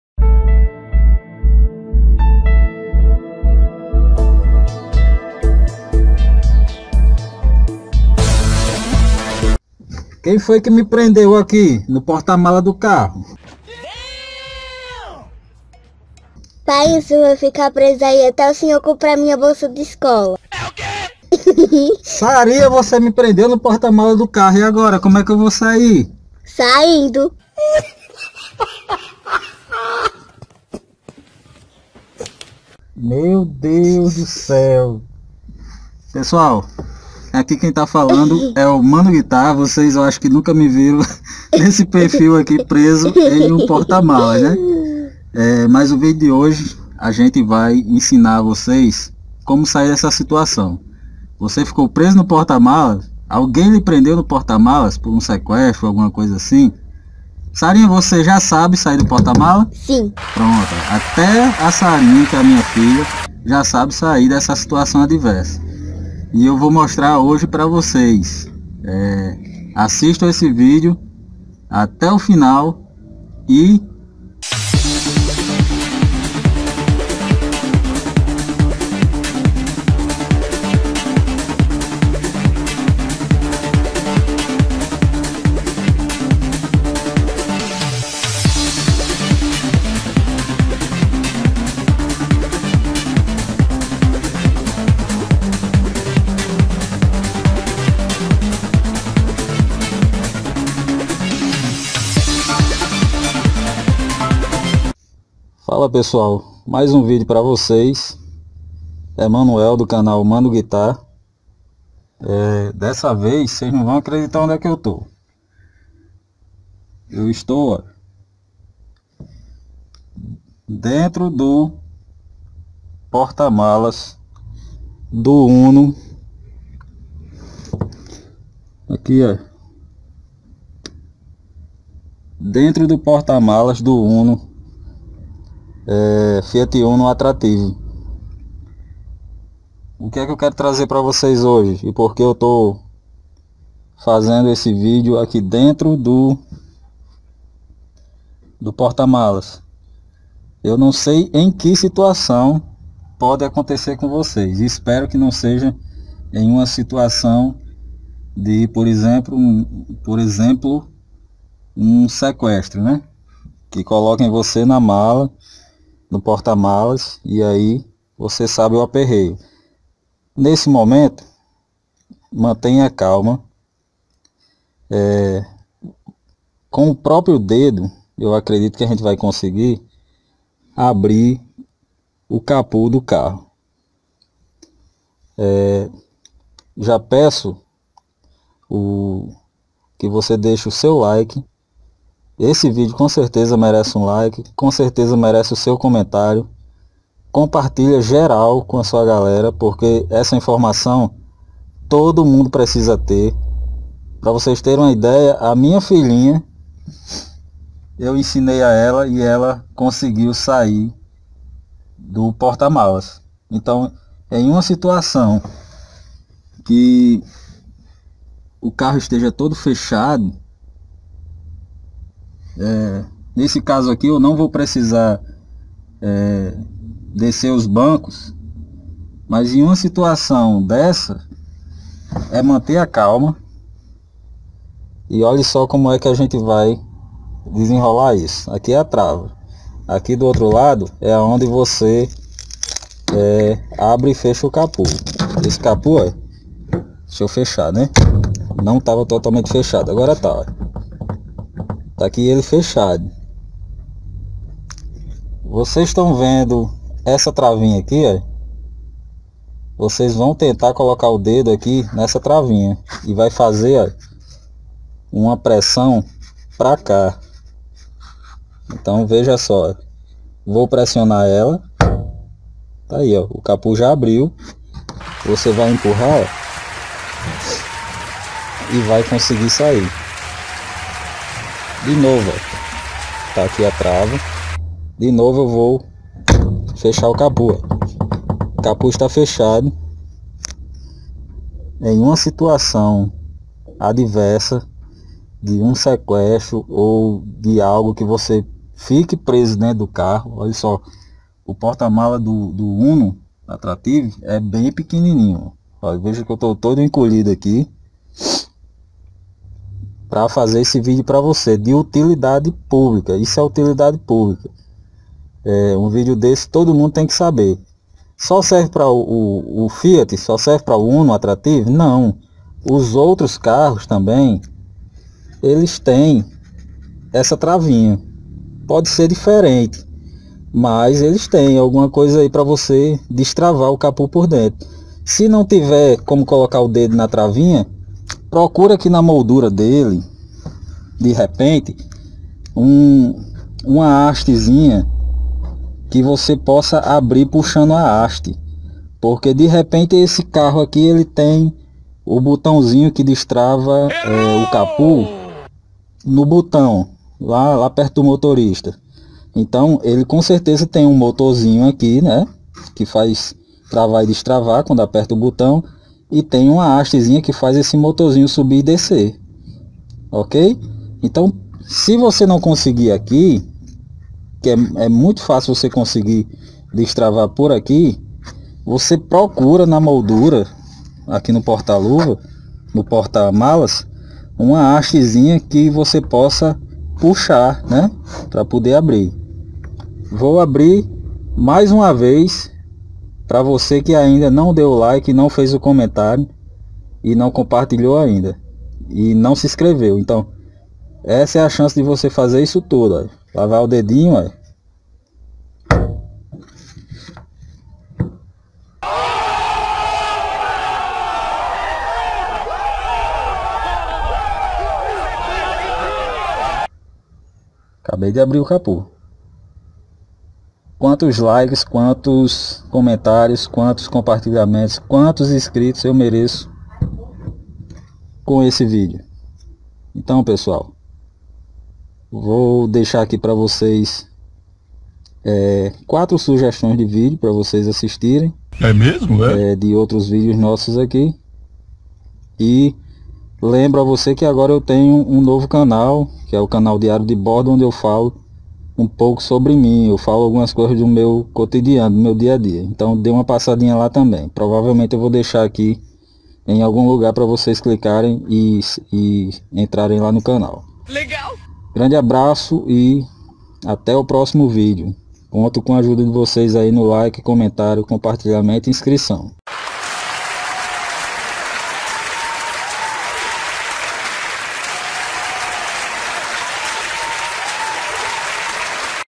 Fiquei preso no porta-malas! E agora?
Criamos uma situação hipotética de uma pessoa em condição de sequestro, presa no porta-malas do carro (fiat uno attractive 2019), tendo que manter a calma, e procurando como abrir o porta-malas por dentro...   ...ficou curioso(a)? então chega mais! porém, já deixa seu "like", seu "comentário", "compartilha" com sua galera, e principalmente, "inscreva-se" no nosso canal.